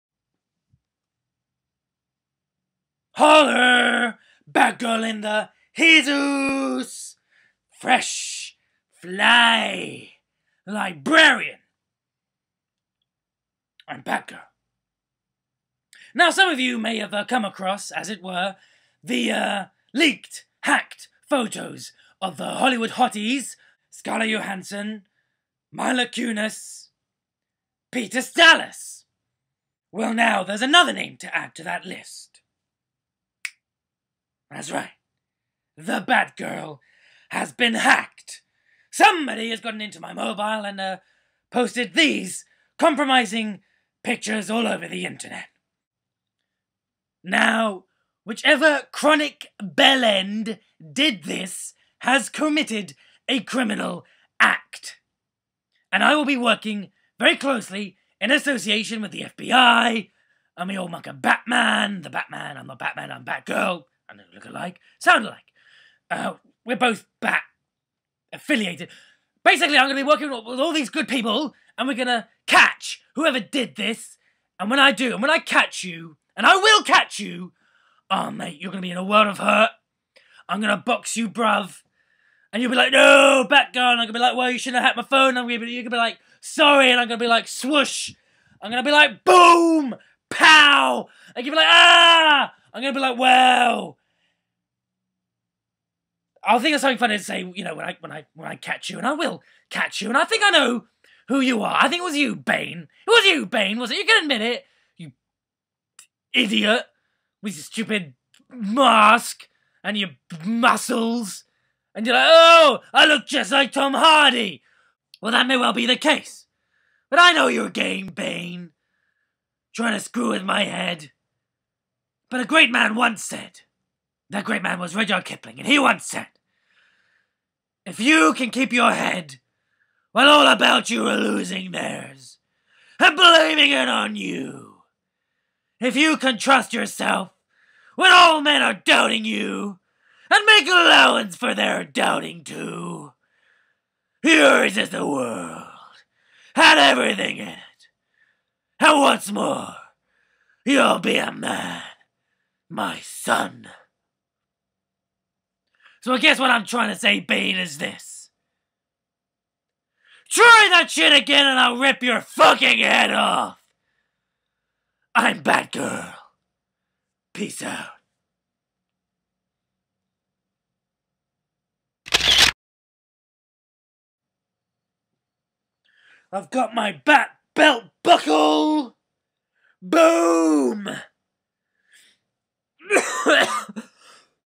Sexy BatGirl Photos leaked!!
Bat Girl reacts to having her mobile hacked and revealing pictures posted on the internet.